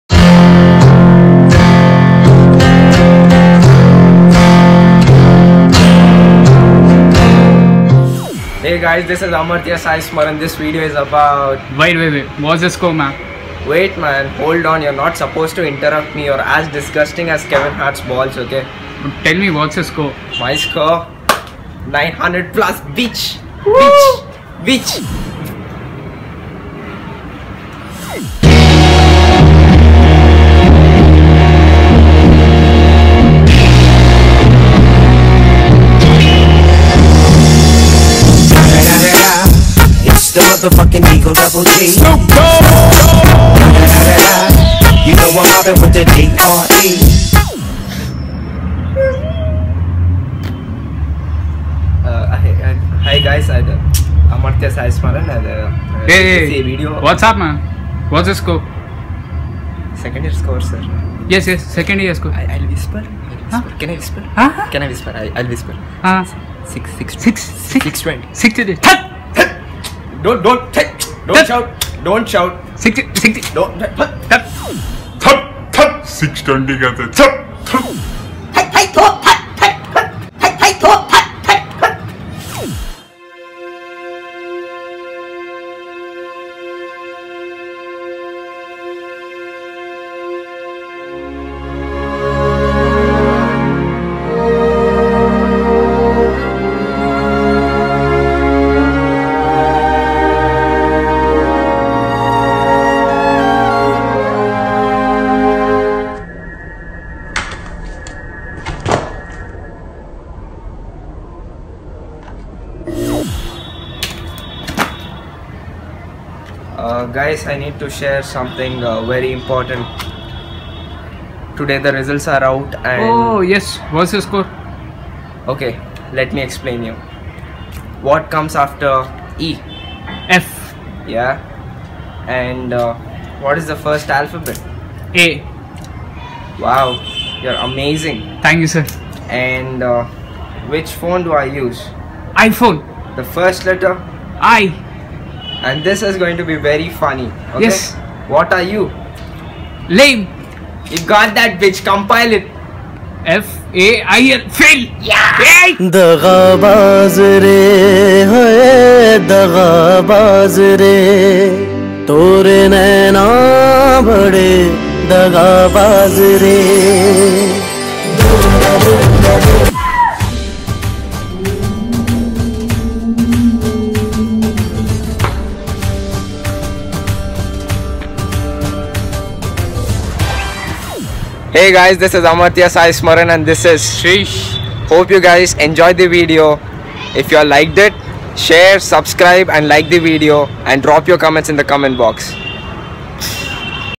"LIFE AFTER RESULTS " BY THE SPOT MAKERS ✌🏼️
Hello everyone, this is the first ever video of the spot makers. This particular video has been shot to show how people celebrate on their success and failure as well and how people treat them based on their success and failure.This applies to all the students in India , especially the" HYDERABADI STUDENTS " who have just got their intermediate results.